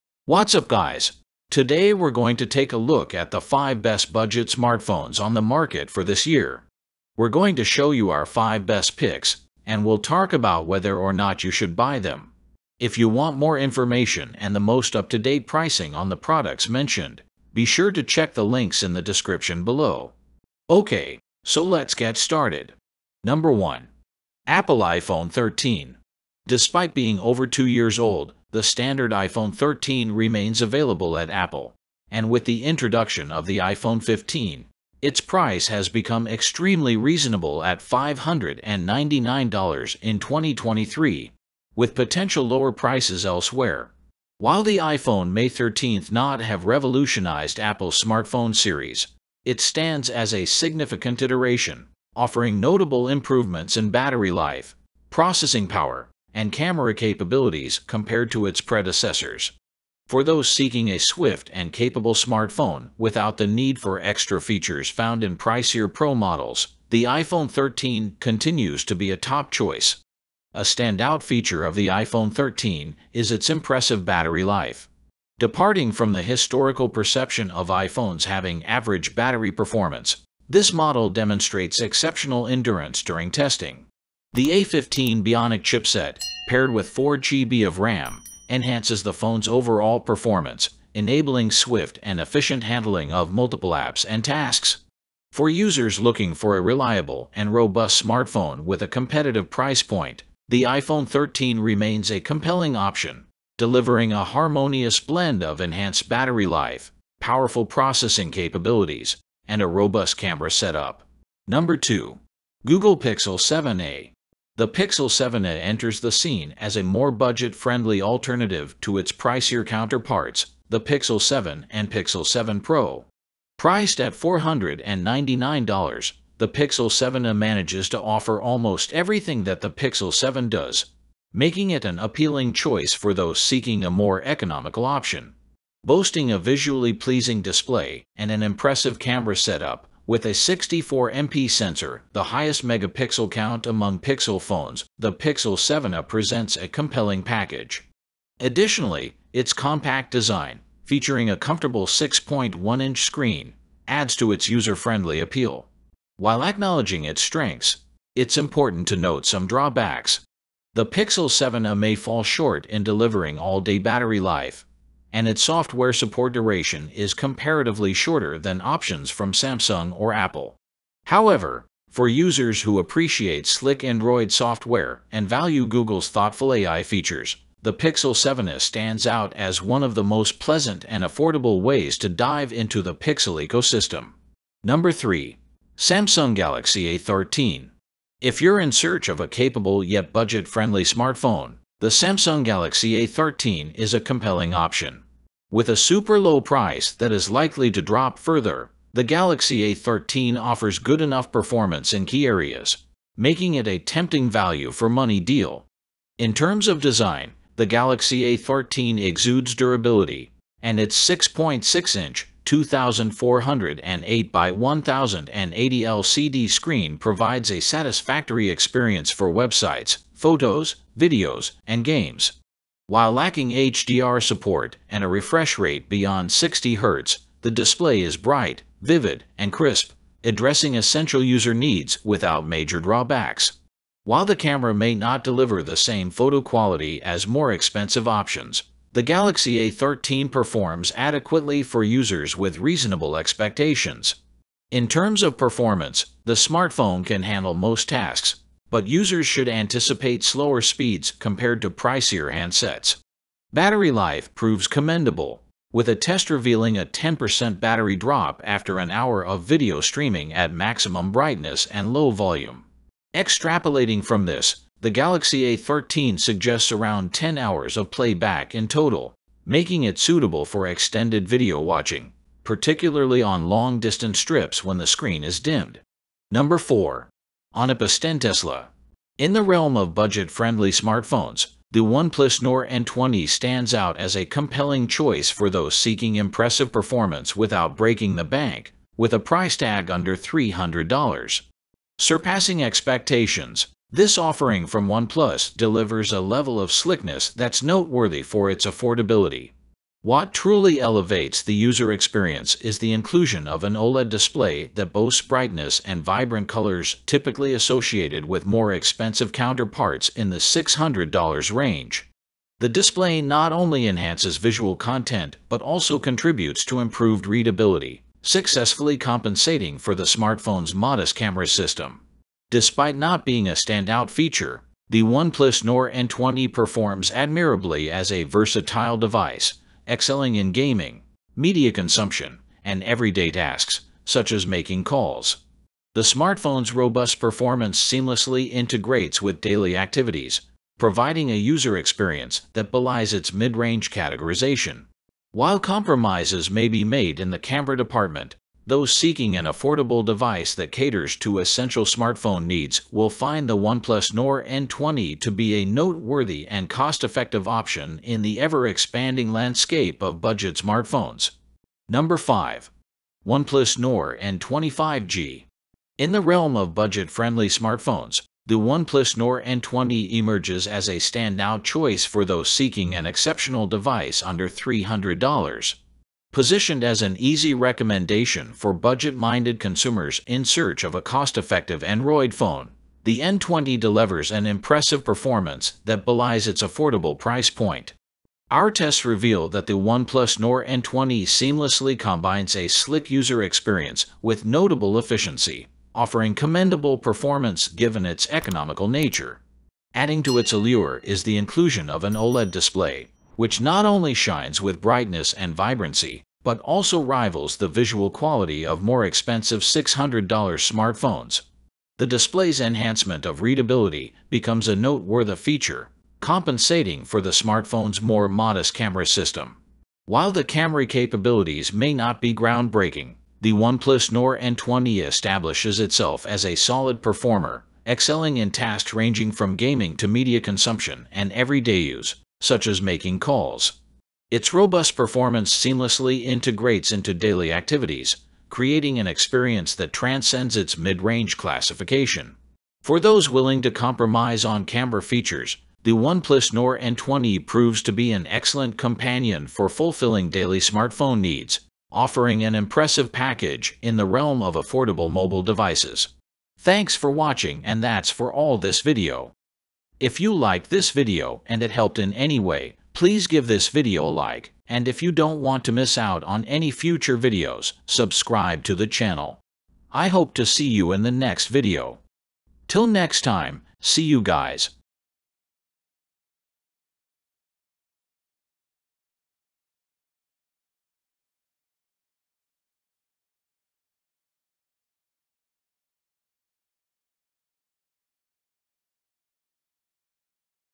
BEST Budget Smartphones in (2024) - Top 5 Best Budget Phone
Best Budget Smartphones in 2024 -  are reviewed in detail in this video. I have also covered the Best Budget Smartphones reviews. So it's a must-watch video for you if you are planning to buy a new Budget Smartphones.

Product Item Check Below :

Apple iPhone 13
Google Pixel 7a
Samsung Galaxy A13
OnePus 10T
OnePlus Nord N20 5G